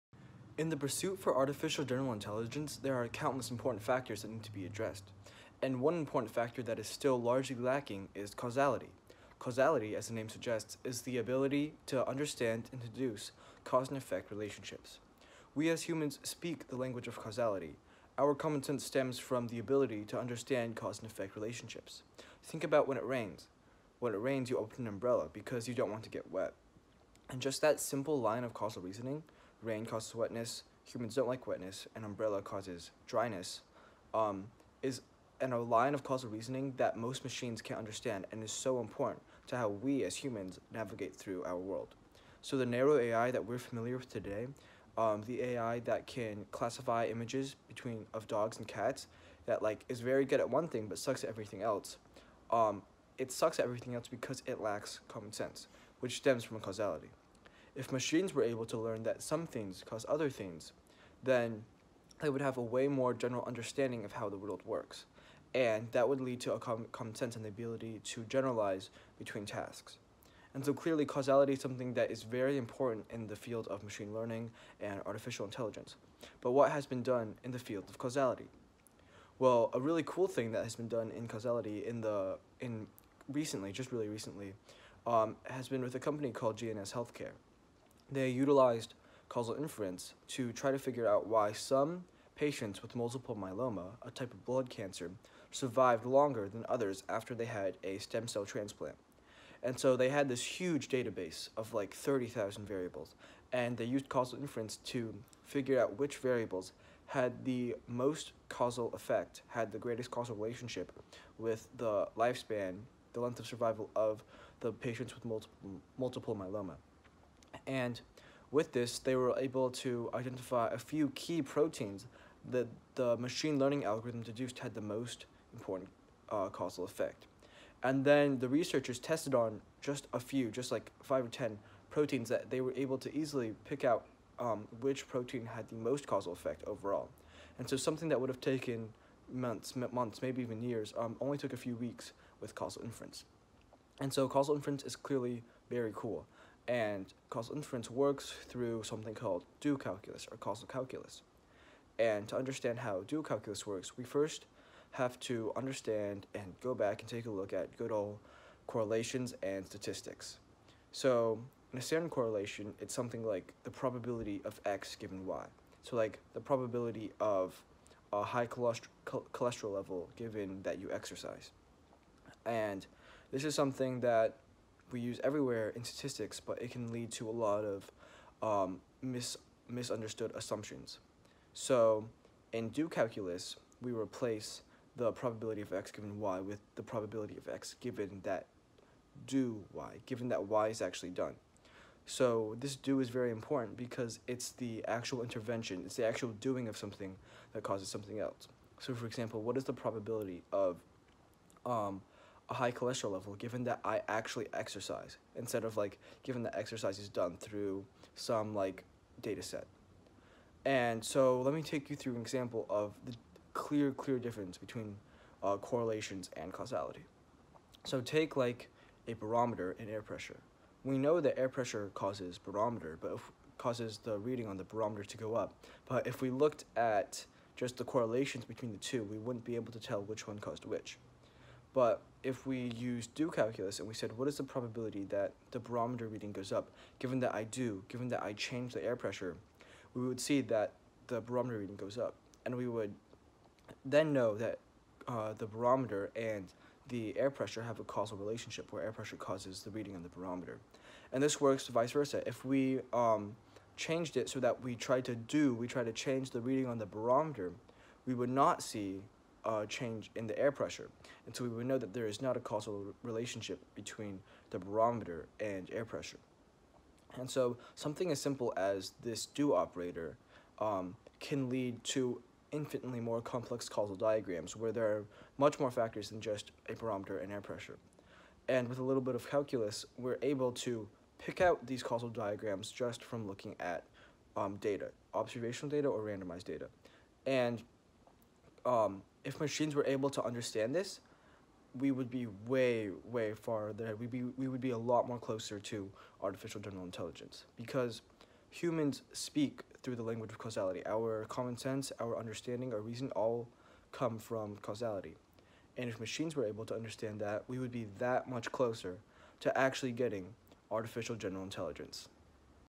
An Intro to Do-Calculus
A really quick and simple intro to causality and do-calculus!